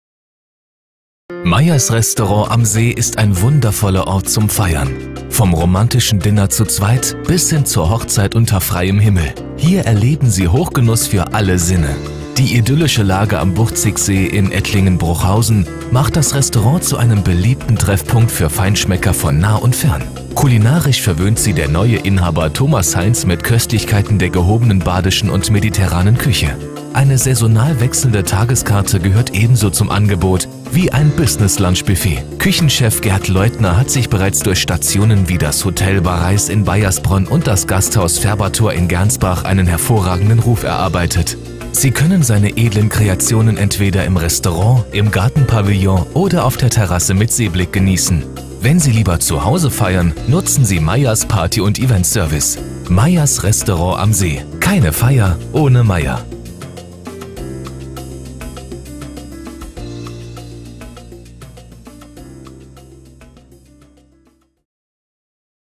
Maier's Restaurant am See in Ettlingen, Landkreis Karlsruhe - Gasthaus und Partyservice
Zu Maier's Restaurant am See aus Ettlingen im Landkreis Karlsruhe finden Sie ein ausführliches Porträt

Maier’s Restaurant am See ist ein wundervoller Ort zum Feiern! Vom romantischen Dinner zu zweit bis hin zur Hochzeit unter freiem Himmel – hier erleben Sie Hochgenuss für alle Sinne!

Die idyllische Lage am Buchtzigsee in Ettlingen-Bruchhausen im Landkreis Karlsruhe macht das Restaurant zu einem beliebten Treffpunkt für Feinschmecker von nah und fern. Kulinarisch verwöhnt Sie der neue Inhaber der Gaststätte Thomas Heinz mit Köstlichkeiten der gehobenen badischen und mediterranen Küche. Eine saisonal wechselnde Tageskarte gehört ebenso zum Angebot des Gasthauses in Ettlingen wie ein Business-Lunch-Buffet.

Küchenchef Gerd Leuthner hat sich bereits durch Stationen wie das Hotel Bareiss in Baiersbronn und das Gasthaus Färberthor in Gernsbach einen hervorragenden Ruf erarbeitet. Sie können seine edlen Kreationen entweder im Restaurant, im Gartenpavillon oder auf der Terrasse mit Seeblick genießen. Wenn Sie lieber zu Hause feiern, nutzen Sie Maier’s Partyservice und Eventservice!

Gasthaus Maier’s Restaurant am See – keine Feier ohne Maier!

Weitere Infos zur Gaststätte Maier’s Restaurant am See in Ettlingen im Landkreis Karlsruhe finden Sie

Unternehmensgründung:
Das Restaurant wurde vor über 20 Jahren eröffnet und am 1.7.2013 von Thomas Heinz übernommen.

Mitarbeiterzahl:
4 Mitarbeitern/-innen unterstützen den Inhaber, der stets um das Wohl seiner Gäste bemüht ist.

Einzugsgebiet Kunden/-innen oder Gäste:
Die Gäste kommen vorwiegend aus Karlsruhe und Umgebung.

Leistungen, Produkte und/oder Spezialisierung:
Maier’s Restaurant am See bietet seinen Gästen gehobene badisch-mediterrane Köstlichkeiten in südländischem Flair. Der aus dem Gasthaus Färbertor in Gernsbach bekannte Küchenchef, Herr Leutner, verwöhnt mit einer saisonal wechselnden Tageskarte sowie mit einem hervorragenden Businesslunch-Buffet die Gaumen der Gäste.
Das Restaurant kann auch für Feiern aller Art reserviert werden.

Erfolgsfaktoren:
In der Küche wird großer Wert auf die Qualität der Zutaten gelegt. Alle Speisen werden stets frisch und mit viel Liebe zum Detail zubereitet, denn die Zufriedenheit der Gäste steht für Herrn Heinz und sein Team an oberster Stelle!

Erwähnenswertes:
Der Blick vom stilvoll eingerichteten Restaurant auf den See lässt Urlaubsgefühle aufkommen.
Das Restaurant bietet für bis zu 80 Personen Platz und ist ein äußerst beliebter Ort für Feiern aller Art.
Parkplätze sind ausreichend vorhanden, die nächste Stadtbahnhaltestelle ist zirka 400 Meter entfernt.

Zahlungsarten:
Die Gäste können bar und mit der EC-Karte bezahlen.

Öffnungszeiten:
Mittwoch bis Freitag: von 12:00 bis 14:30 Uhr und von 17:30 bis 24:00 Uhr
Samstag: von 17:30 bis 24:00 Uhr
Sonntag: von 11:30 bis 14:30 Uhr und von 17:30 bis 24:00 Uhr
Montag und Dienstag Ruhetag.